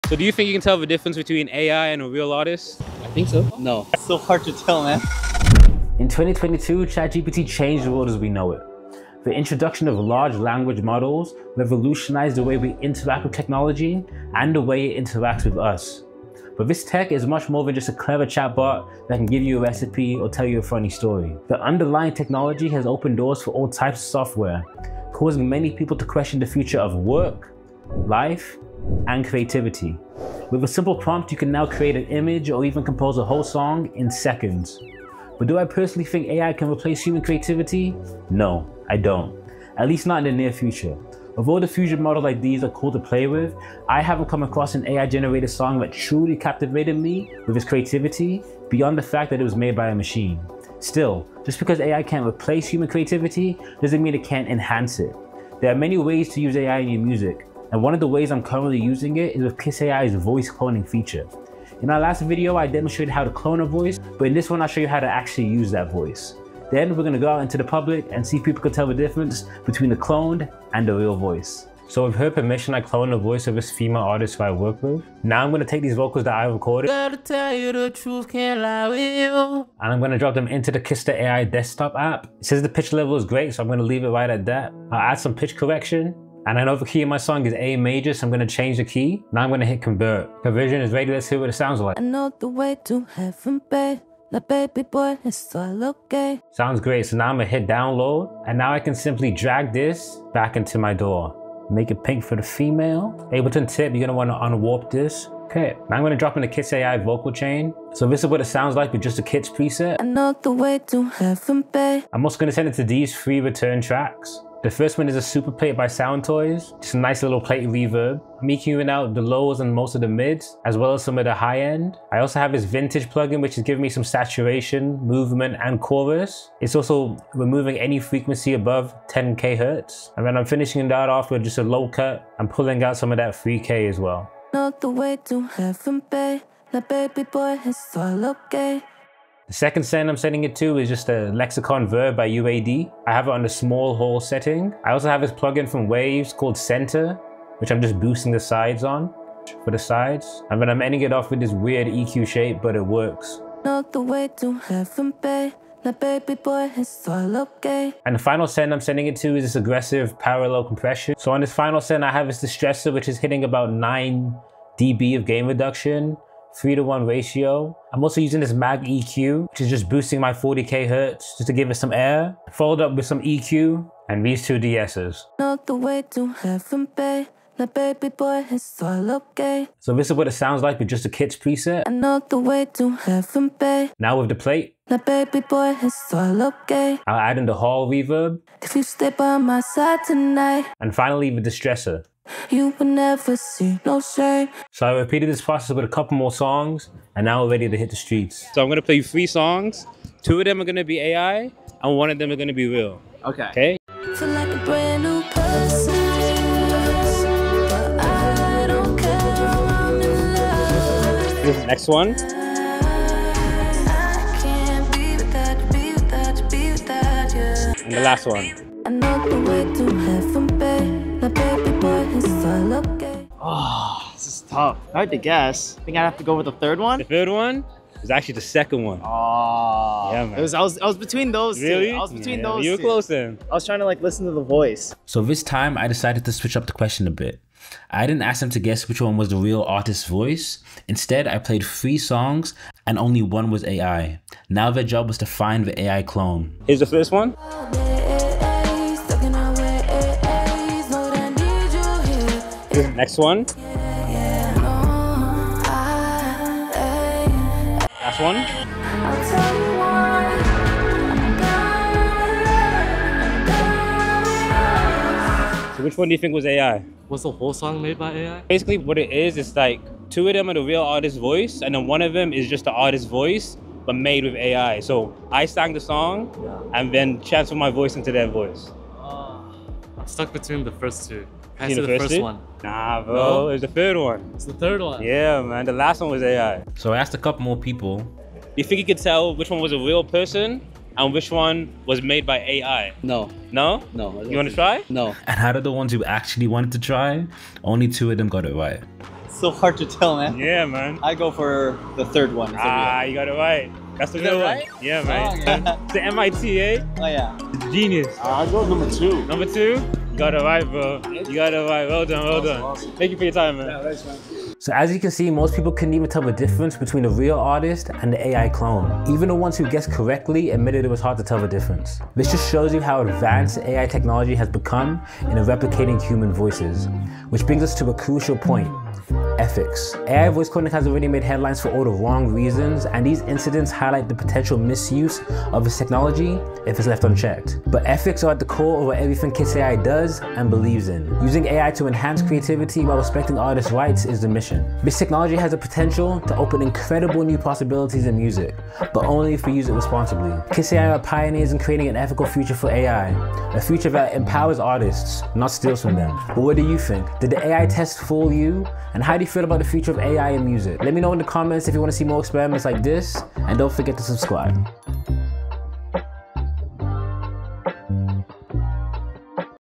AI Voices vs. Real Voices: Kits.ai Hits the Streets
In our last video, we showed you how to clone a voice with Kits.ai. In this video, we’ll guide you through processing Kits vocals step-by-step. Then, we hit the streets to see if people can tell the difference between the AI-generated voice and the real one. Spoiler: Most people can’t! Discover how Kits.ai delivers lifelike results that blur the line between AI and reality.

00:00 - Intro
01:10 - Vocal Cloning Demonstration
01:46 - Processing Your Vocals
02:59 - Before vs. After
03:15 - Real vs. AI Voice Test 1
03:54 - Real vs. AI Voice Test 2
05:14 - Key Takeaways
05:27 - AI Ethics
05:45 - Conclusion